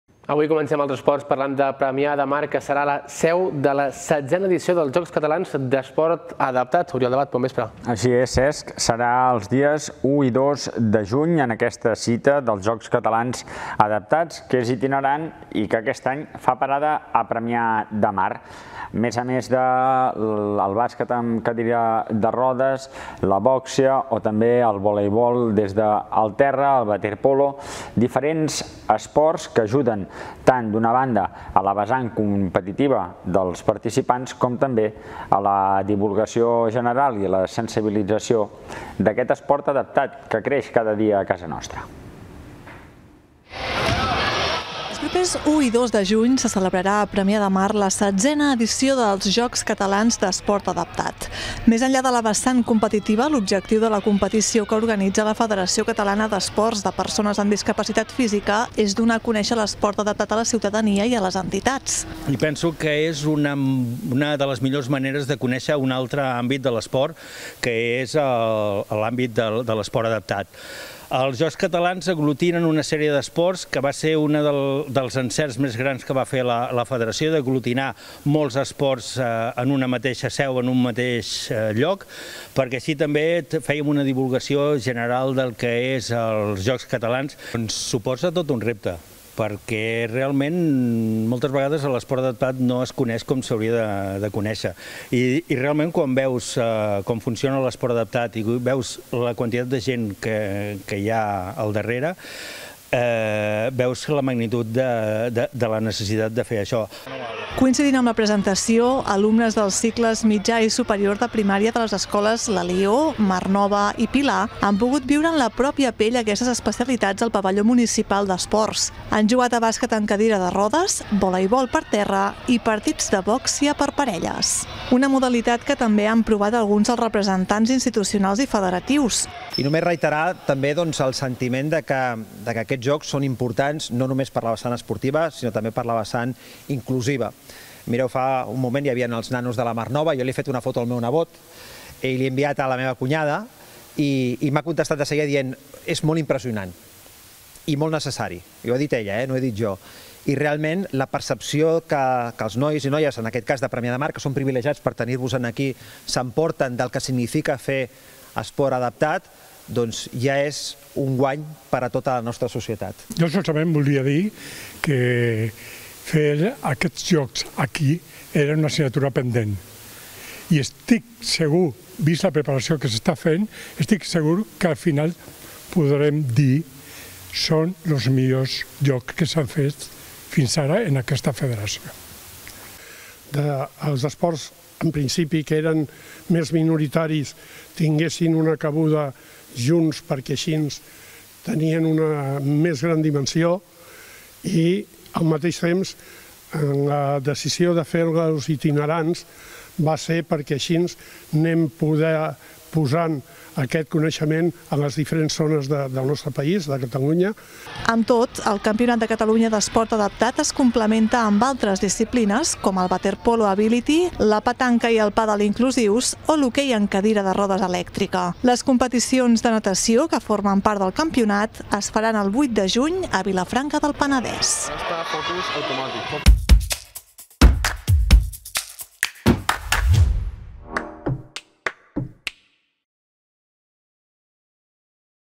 Premià de Mar, referent català de l'esport adaptat
El municipi maresmenc serà la seu de la setzena edició dels Jocs Catalans d'Esport Adaptat.